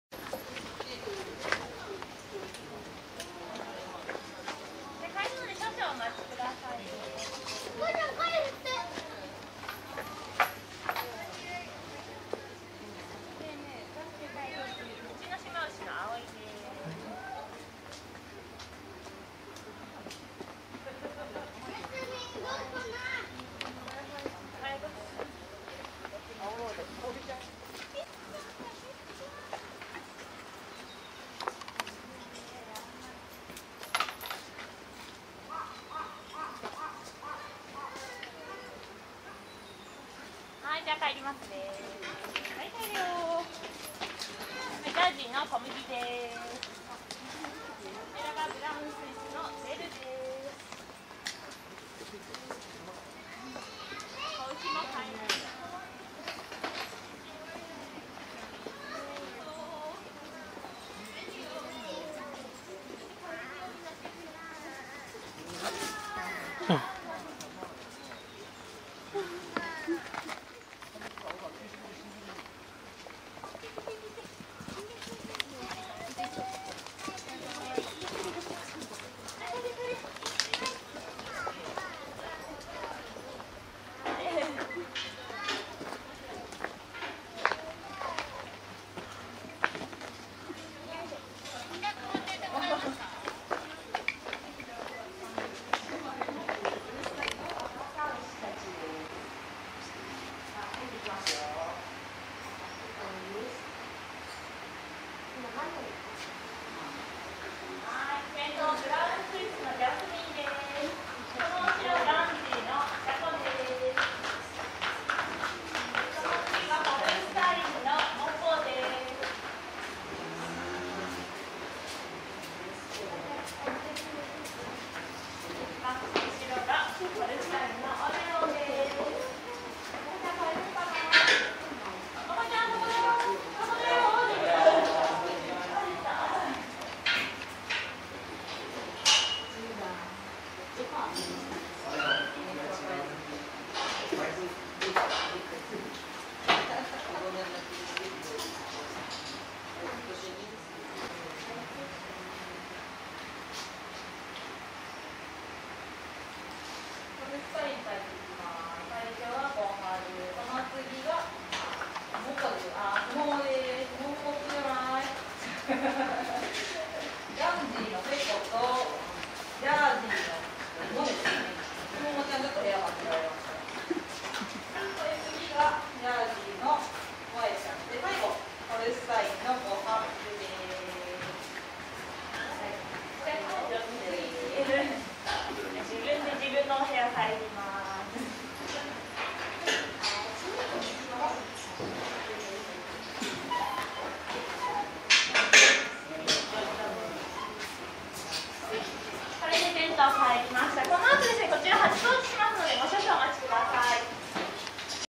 みんな～帰るよ～ おかえり牛さん 埼玉県こども動物自然公園 2023年6月25日
埼玉こども動物自然公園
2023年6月25日